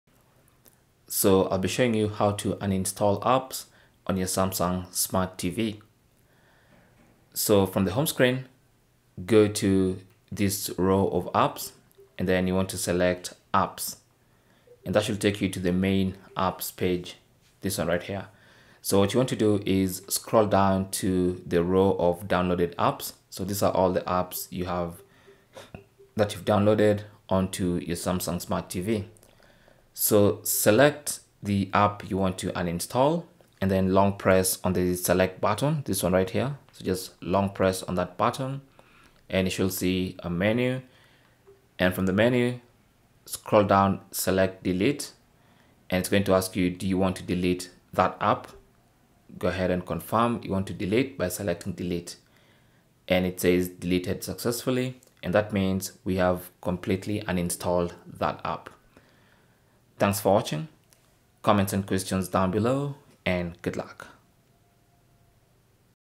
How To Uninstall Apps On Samsung Smart TV
Learn How To Uninstall Apps On Samsung Smart TV.

Looking to clear up some space on your Samsung Smart TV by removing unnecessary apps? You're in the right place! In this tutorial, we'll walk you through the simple steps to uninstall apps from your Samsung Smart TV, whether you're using a newer model with Tizen OS or an older one with the Smart Hub interface.

Uninstalling apps on your Samsung Smart TV is a breeze once you know how to do it. Whether you want to remove pre-installed apps or ones you've downloaded yourself, we've got you covered. Follow along as we demonstrate the process, step by step, making it easy for you to tidy up your TV's app library and optimize its performance.

1. Navigate to the home screen. From here, choose the option Apps. This will take you to the Apps store. Use you remote to navigate to the section labelled Downloaded Apps.

2. Ensure that the app you want to uninstall has been highlighted, then, long press on the select button on your remote. From the pop up that appears, choose Delete.  Confirm your choice. The app will then be uninstalled.

Cell Phone Tripod Adapter